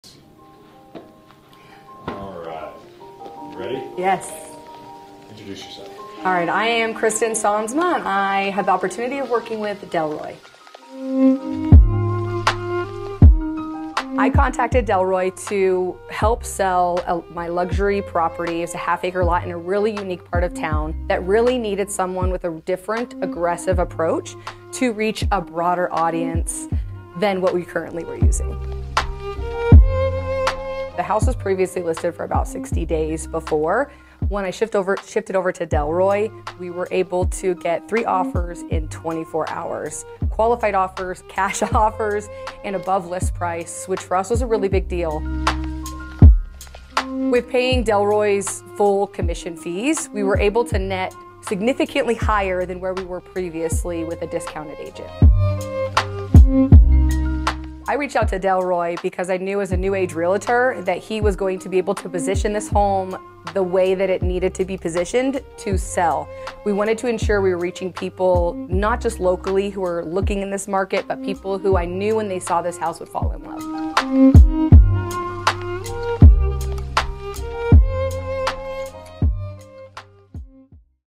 New Testimonial Video Alert: How Delroy Transformed My Property Sale by Kristen Sosma
🏡 Ever wondered what makes a real estate transaction truly exceptional? Dive into our latest YouTube video to find out!

🎥 In this must-watch testimonial, Kristen shares her game-changing experience with Delroy Gill, who is refining the way Denver sells luxury properties. From a half-acre lot in a unique part of town to attracting cash offers above list price, Kristen’s story is nothing short of inspiring.

🚀 Discover how Delroy’s aggressive and innovative approach outperformed traditional methods, leading to three offers in just 24 hours! Yes, you read that right - 24 hours!

💰 Kristen's experience reveals a crucial insight: sometimes paying full commission fees can actually net you more. She explains why choosing Delroy, known for his fresh and effective strategies, was the best decision for her high-end property.

🌐 But there's more! Learn how Delroy’s strategy isn't just about local buyers. He targets those who will fall in love with the property, wherever they are.

👀 Don't miss this opportunity to peek into the world of luxury real estate through the lens of a satisfied client. Whether you're a homeowner, investor, or just curious, this video is for you!